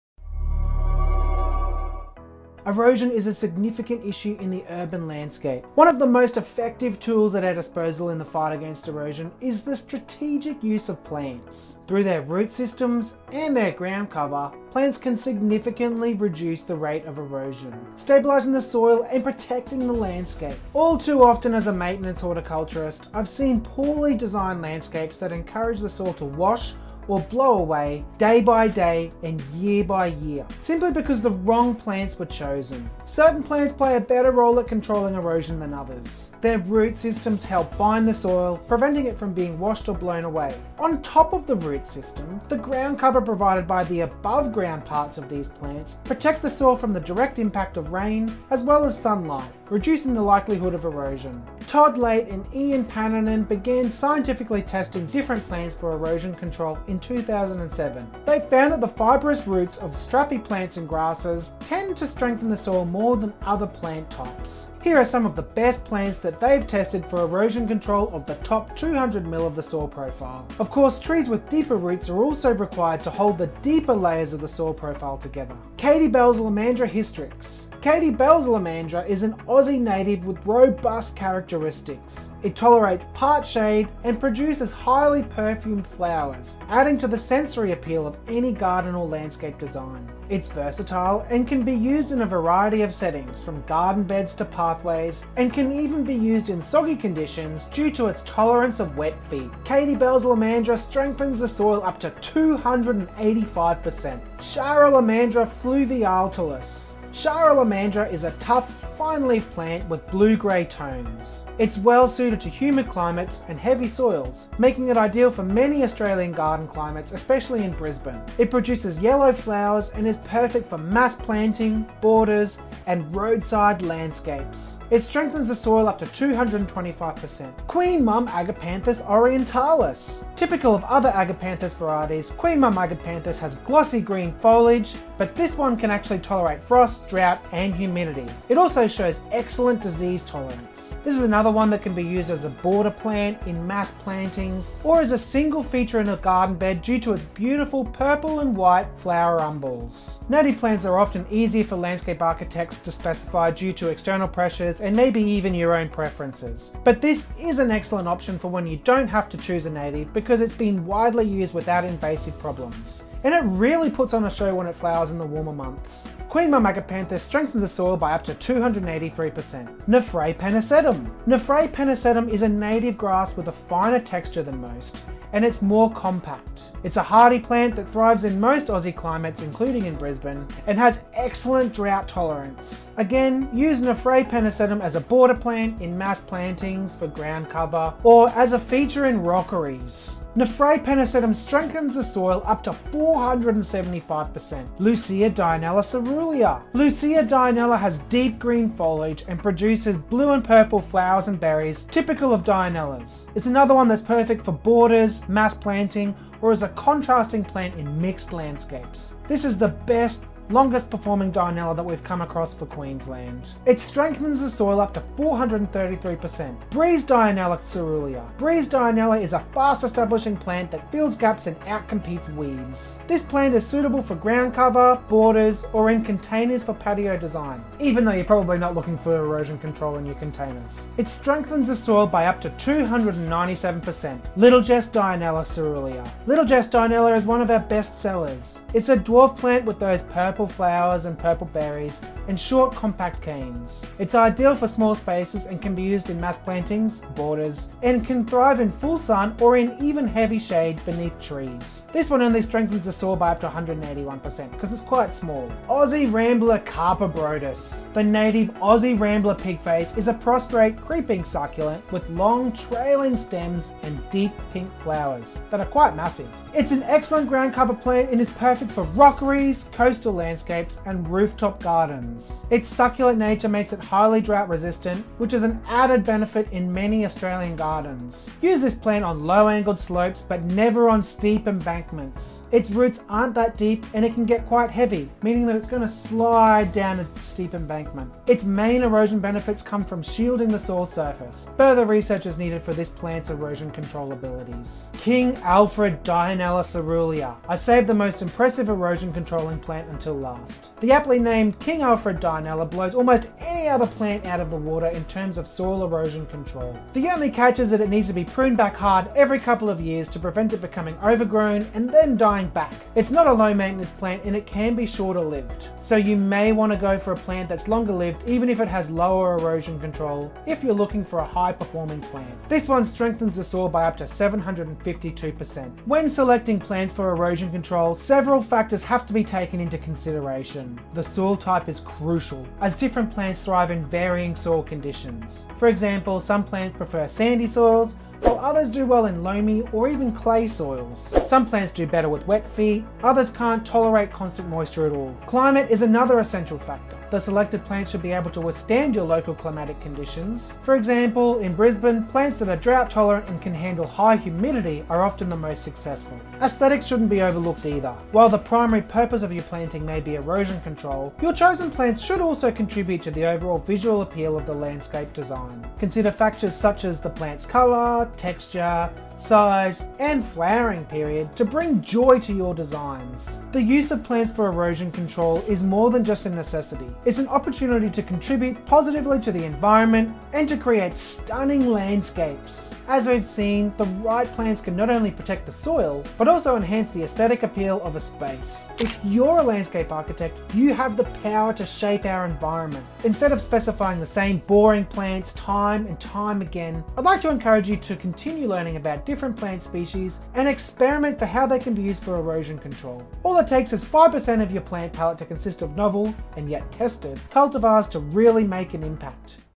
Save Your Soil: The Best Plants for Erosion Control
There's nothing worse than soil erosion. How do you prevent it, and how can you repair soil stability?

In this video, we'll discuss some of the best plants that can hold soil together and protect the soil surface from rain droplets. We'll explore research explaining what percentage of soil stability each plant can provide. Spoiler alert: strappy plants and grasses tend to offer the best protection.